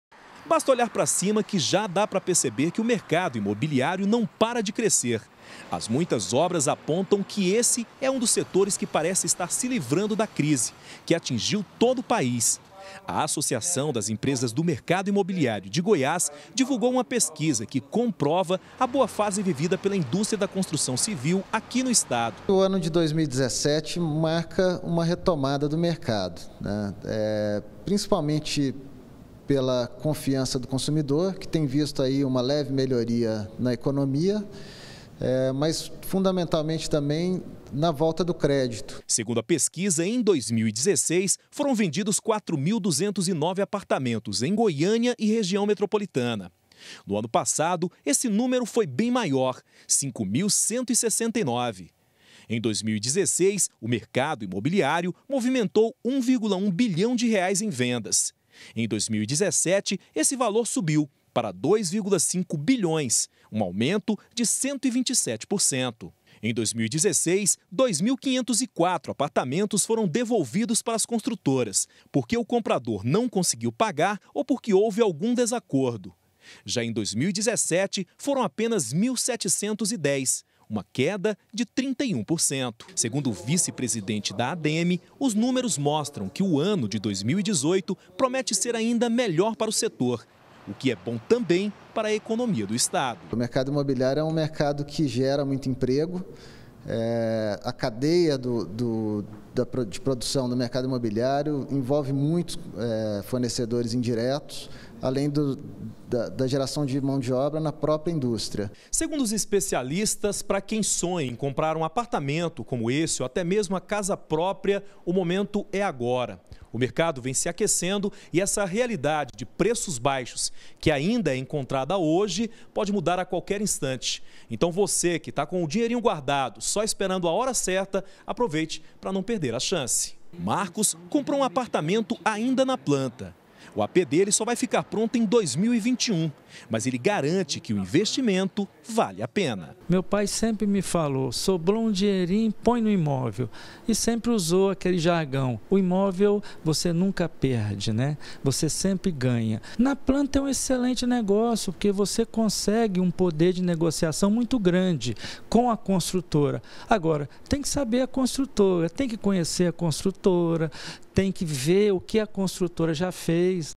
GR - Hora de comprar: Crescem as vendas de imóveis em Goiás - 06-03-2018
E uma boa notícia pra quem está querendo comprar um imóvel. Com a volta do crédito e juros mais baixos, agora é a hora. O setor imobiliário está otimista e acredita que o consumidor está mais confiante. Pra se ter uma ideia as vendas aumentaram em mais de cem por cento no ano passado se comparando com o 2016. E 2018...promete.